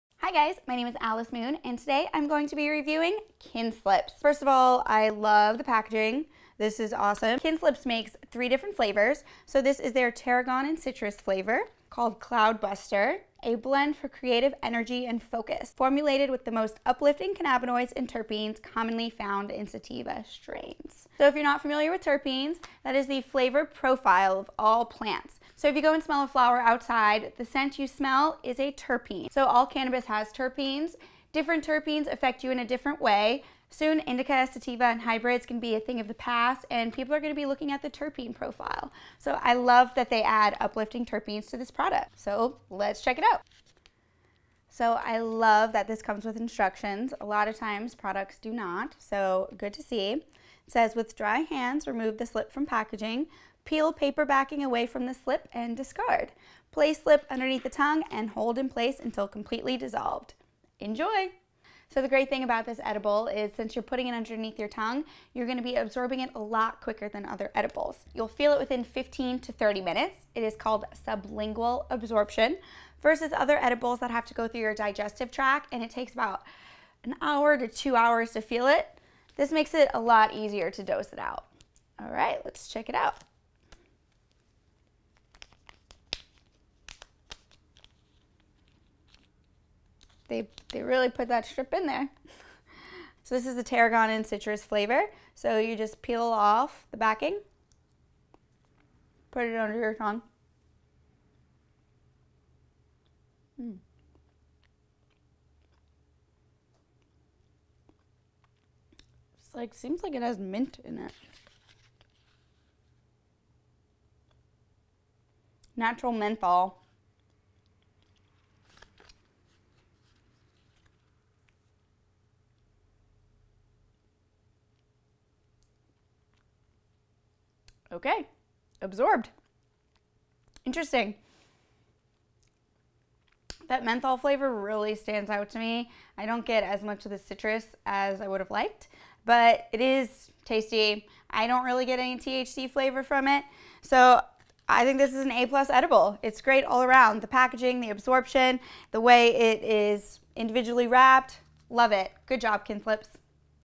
Edible Review: Kin Slips Cloud Buster Strips
My first impression of Kin Slips Cloud Buster Strips

Kin Slips is an award winning edible company based in Los Angeles, California.

What did you think of this live reaction video?